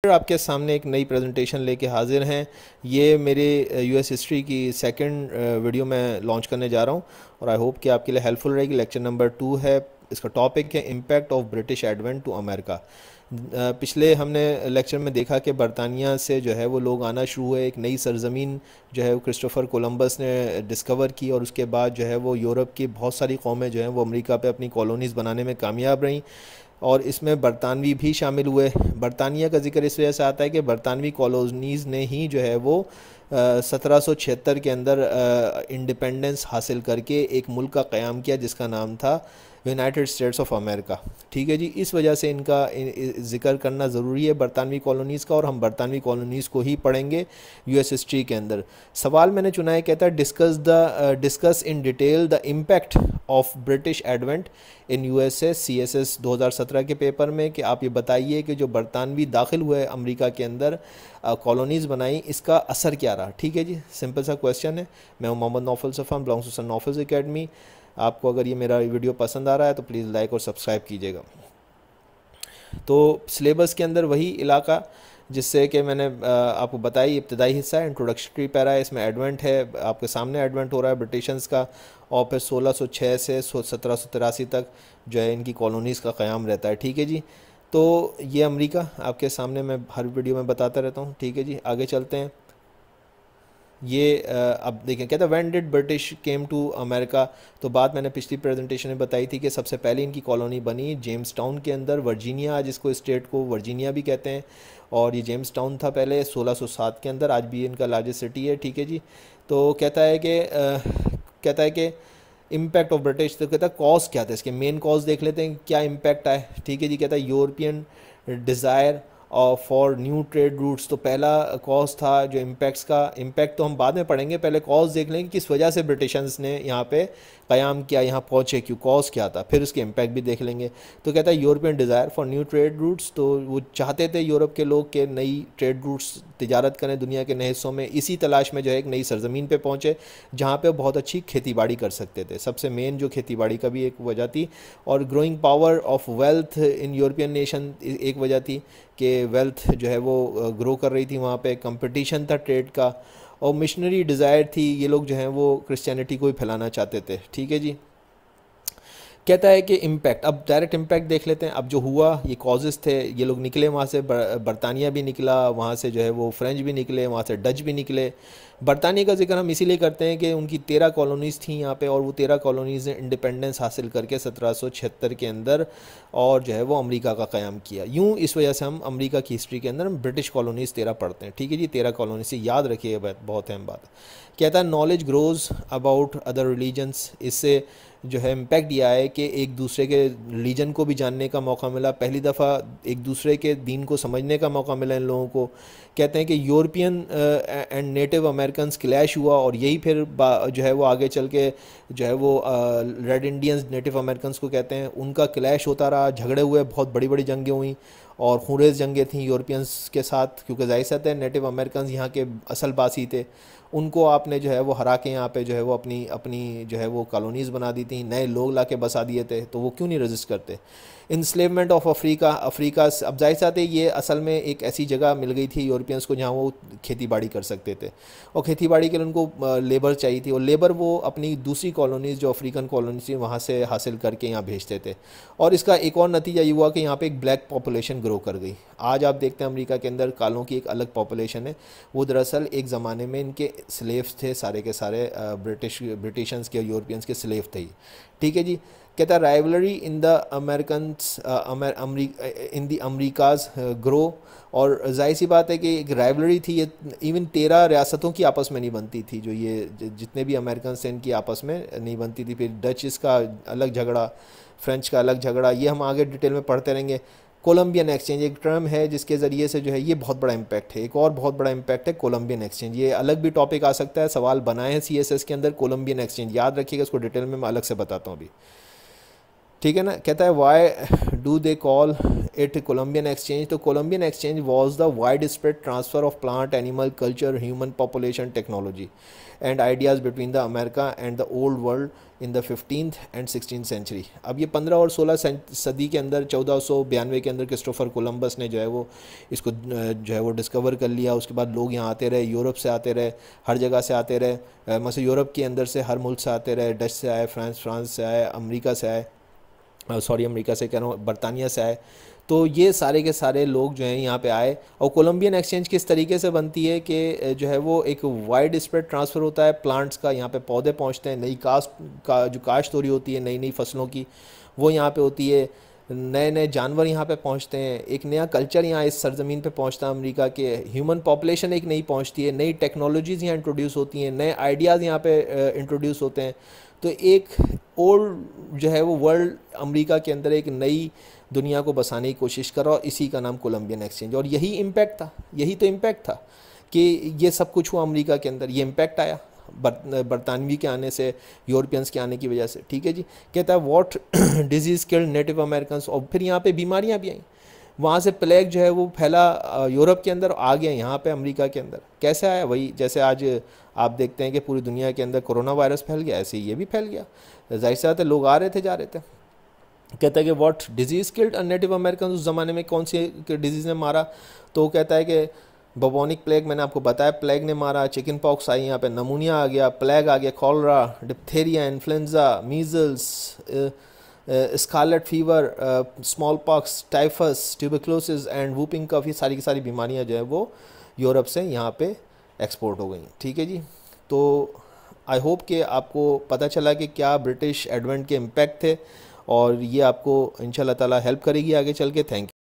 Impact of British Advent in America|Columbian Exchange|Urdu|Hindi|CSS|British Colonization in Americ
The USA as a British Colony (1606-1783) Or British Colonization in America
The English established their first successful, permanent colony in North America at Jamestown in 1607 on the Chesapeake Bay.
The advent of the Europeans to British supremacy. Columbian exchange.

All Lectures are here: باقی لیکچر سیریز یہاں ہیں
Sir Nofil's Academy